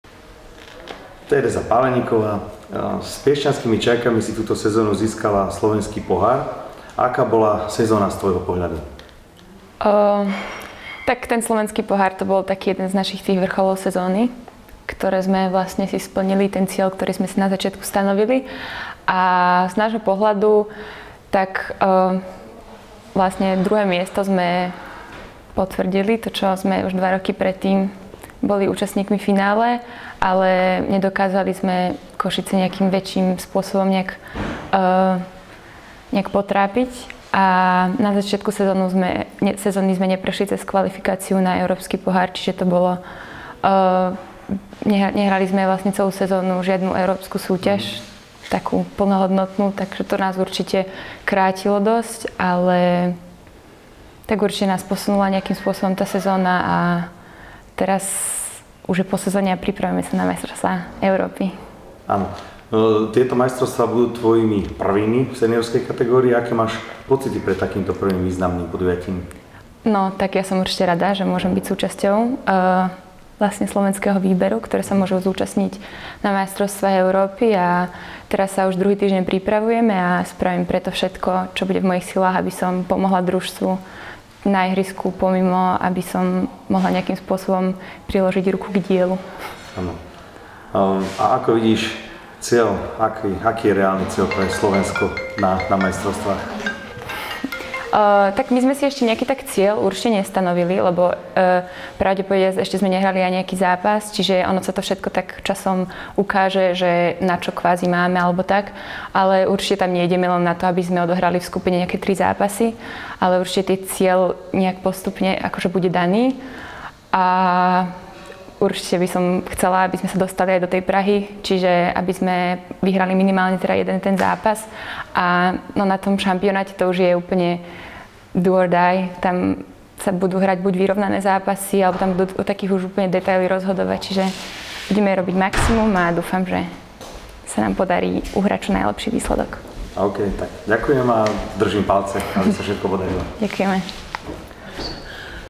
Rozhovor - Tereza Páleníková
O sezóne a prvých Majstrovstvách Európy v seniorskej kategórii s talentovanou hráčkou Piešťanských čajok Terez Páleníkovou.